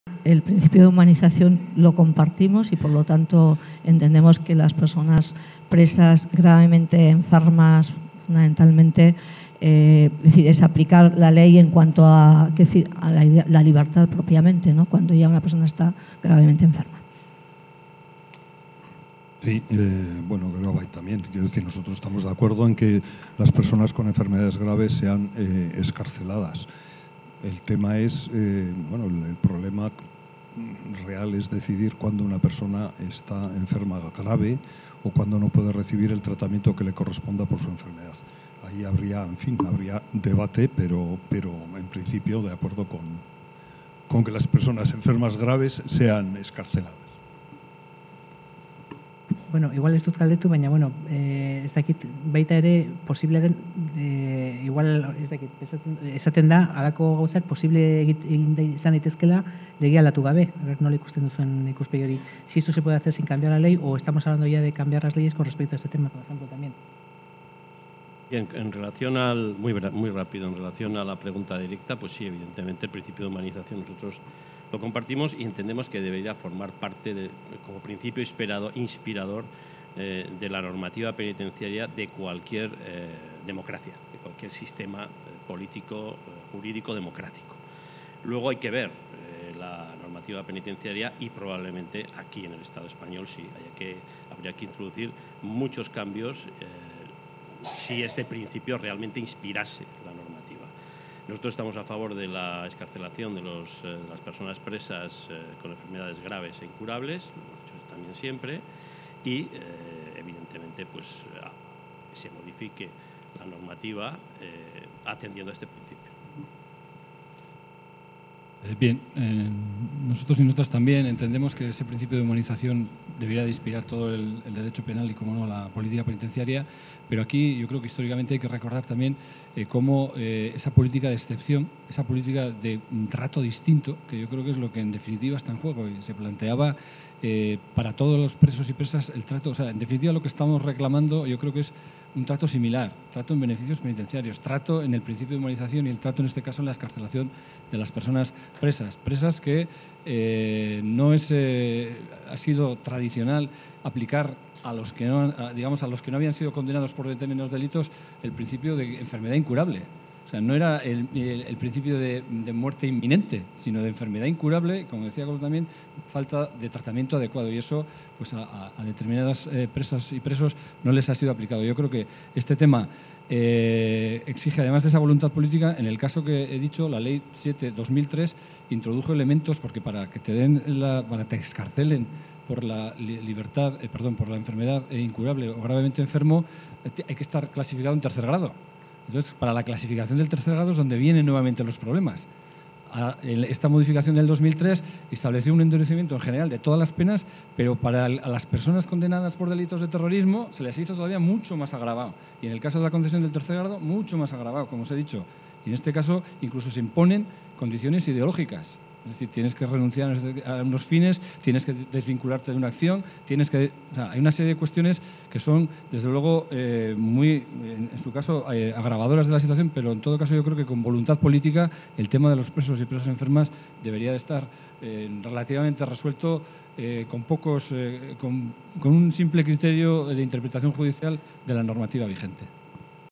HITZORDUAK 2- Presos enfermos y de avanzada edad
Koldo Martinz (Geroa Bai), Adolfo Araiz (EH Bildu), Tere Sáez (Podemos) y Miguel Nuin (E-I), han participado en la mesa redonda organizada por el Foro Sozial bajo el título "Una nueva política penitenciaria para un nuevo tiempo político", que ha tenido lugar en Iruñea.